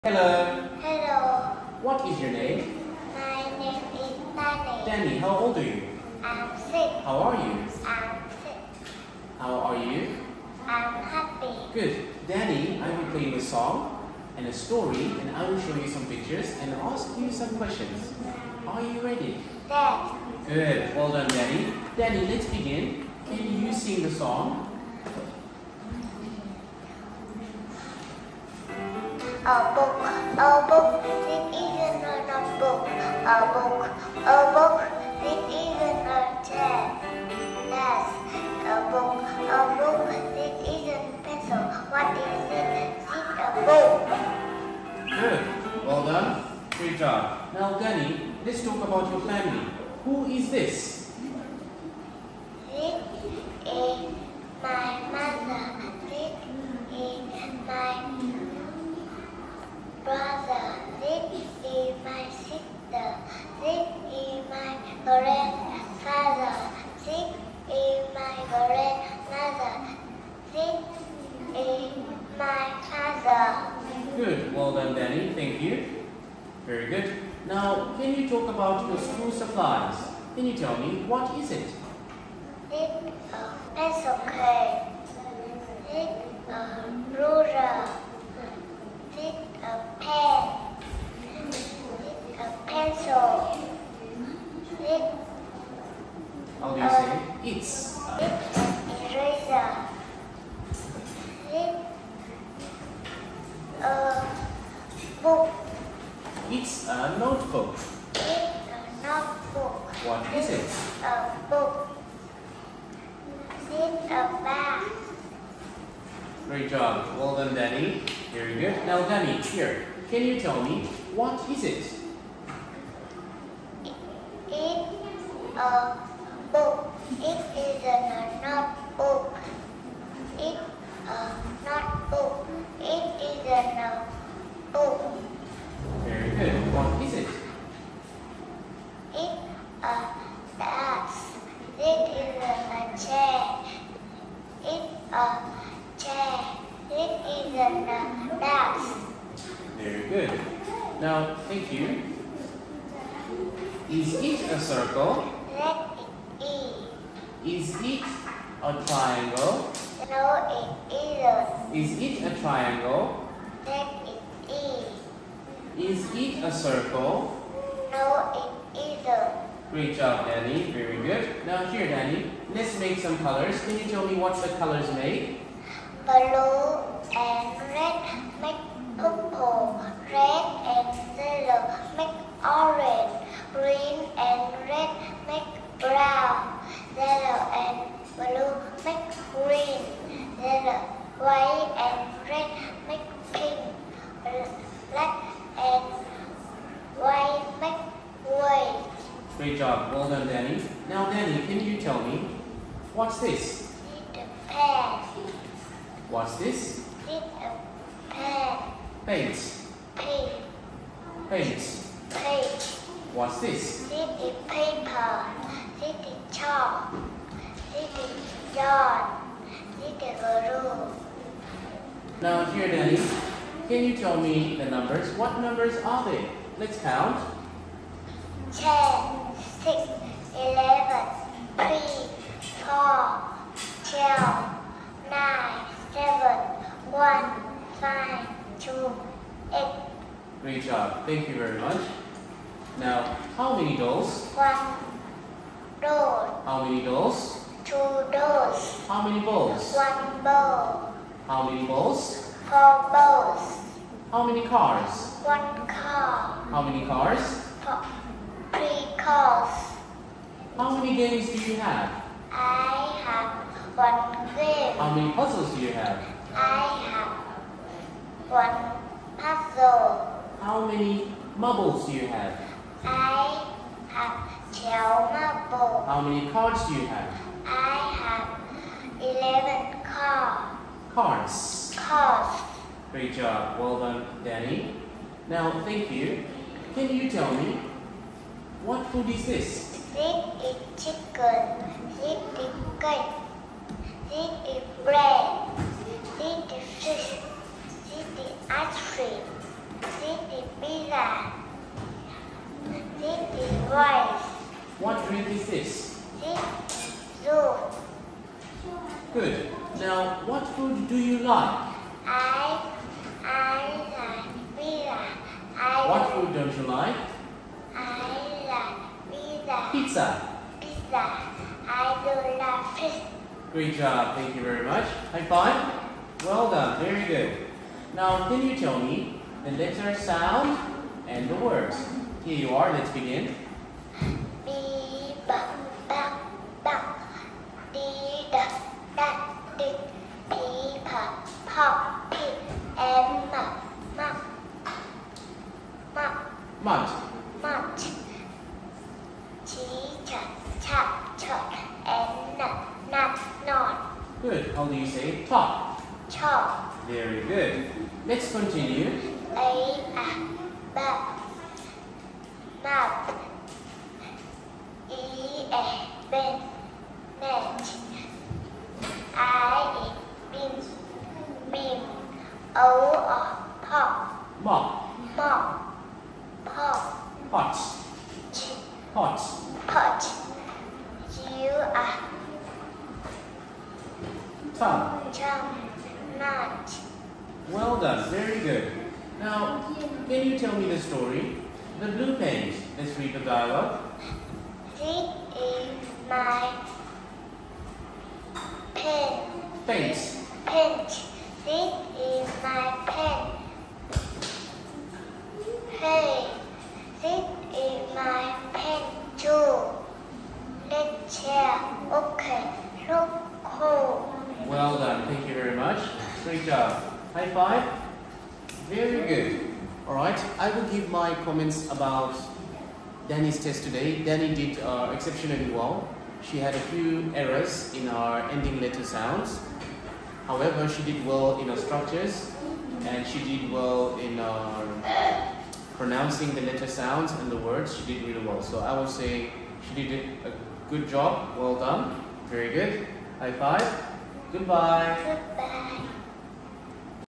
NGUYỄN NGỌC DIỄM - DANIE - BEGINNER B1 - AMASTER
Chúc mừng Dannie đã hoàn thành bài thi của mình nhé!.
Con hát và đọc truyện rất tự tin và tự nhiên.
Con ghi nhớ tốt từ vựng các chủ đề về đồ dùng học tập, nhóm từ gia đình, hình khối kết hợp với màu sắc,số đếm các đồ vật.
Mẫu câu con vận dụng đa dạng các câu: It's.../This is my.../I have.../I like.../I don't like. /...make. / There is one.../There are.
Về phát âm: Con nói có âm đuôi các từ và bật được âm"s" của danh từ số nhiều tốt.
Con lưu ý phát âm từ pencil case, marbles nhé!